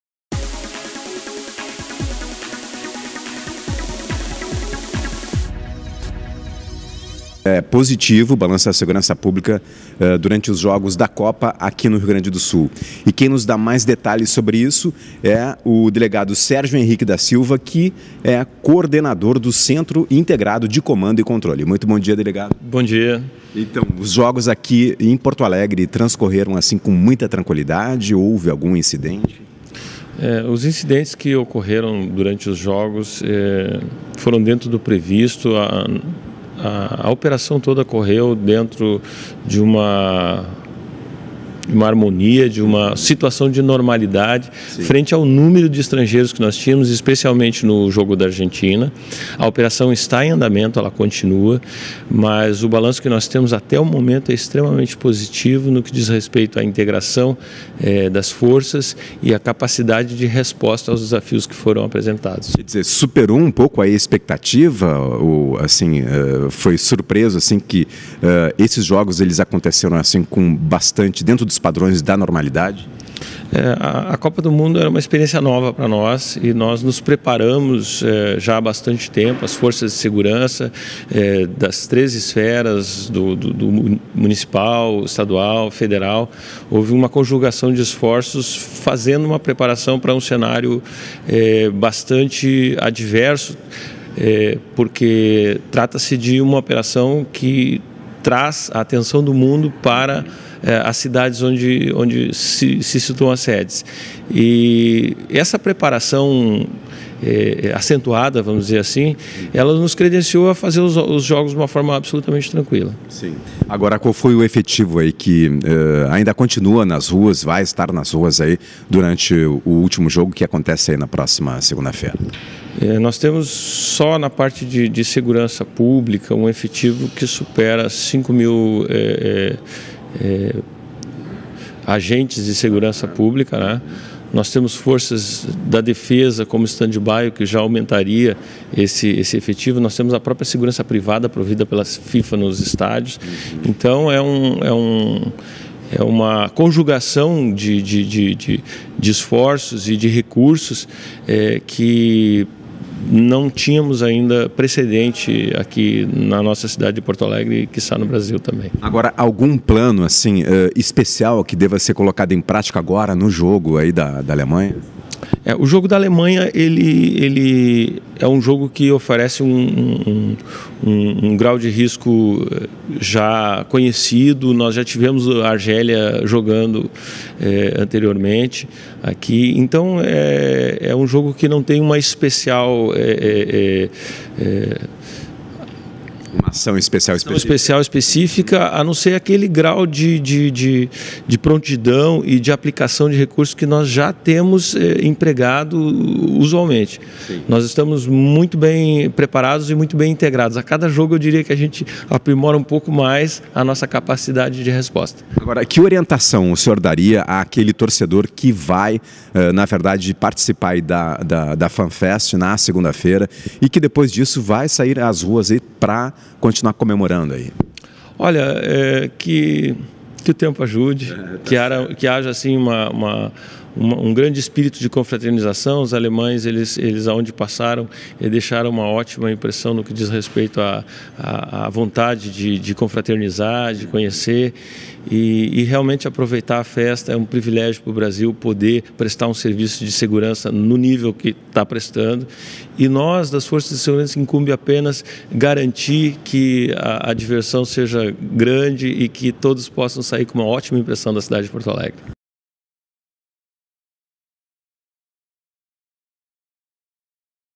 Segurança pública apresenta balanço positivo da operação da Copa
A partida entre Nigéria e Argentina, pela Copa do Mundo Fifa 2014, na última quarta-feira (25), no Estádio Beira Rio, em Porto Alegre, teve algumas ocorrências envolvendo turistas. Os casos registrados, porém, foram de pequena proporção e não atrapalharam a grande festa.